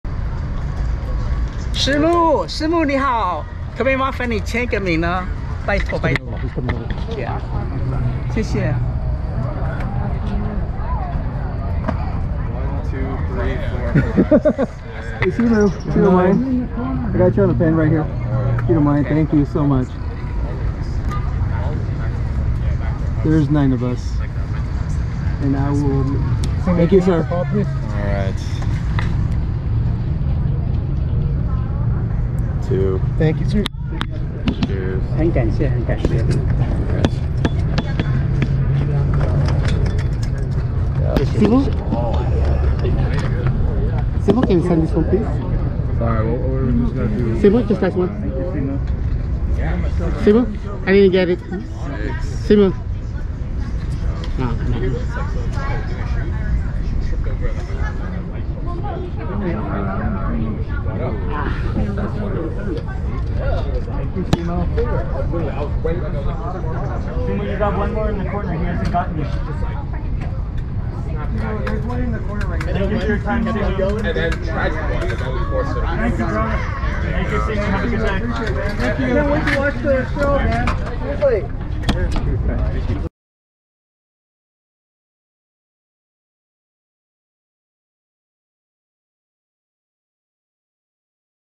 Simu Liu is a little nicer than usual | signing autographs one per person @kimmel live
Simu Liu is a guest host at kimmel live,surprisingly a little nicer and signs one autograph each but ignored my request to sign my poster,better luck next he might be extra nicer.